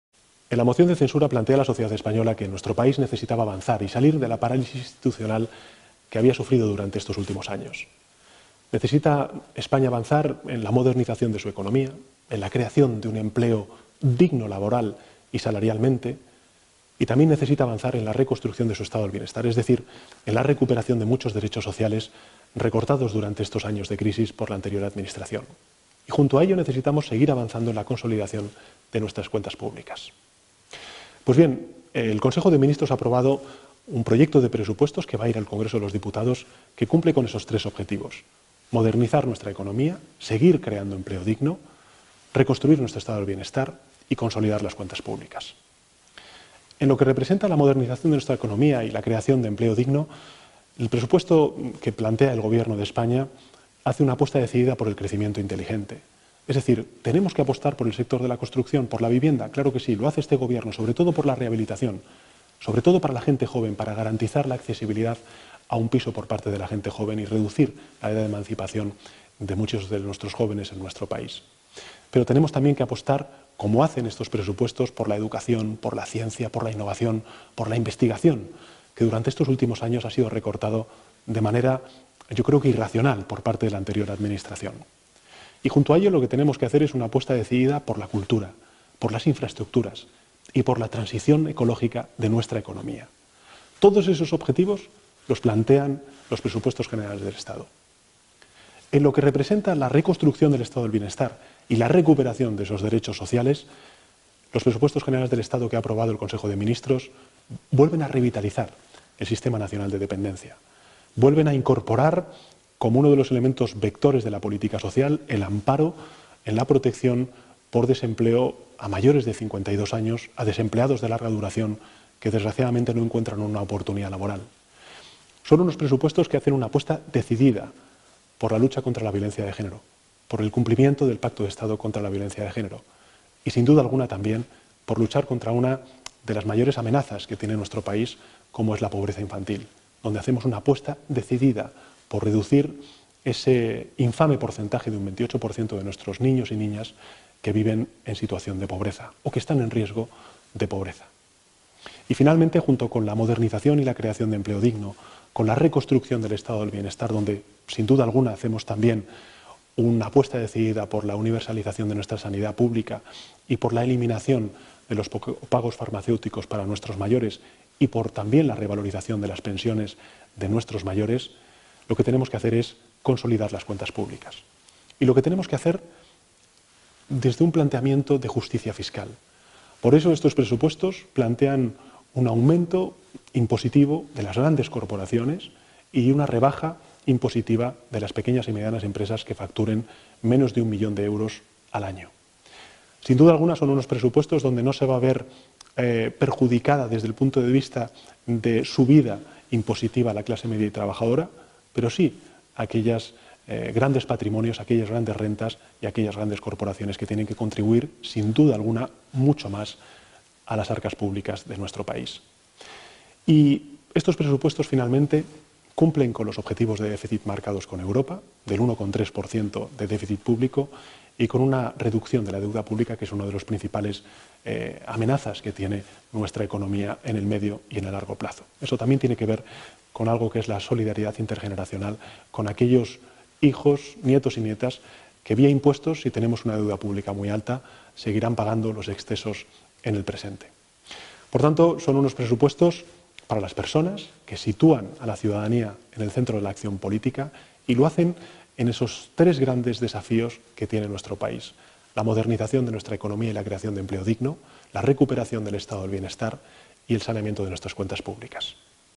Por unos Presupuestos más sociales | Presidente del Gobierno
Los PGE2019 apuestan por la modernización de nuestra economía, la reconstrucción del Estado del Bienestar y la reducción del déficit público, con un objetivo central: las personas, especialmente en las más vulnerables.
Unos presupuestos para una España mejor para todos y todas.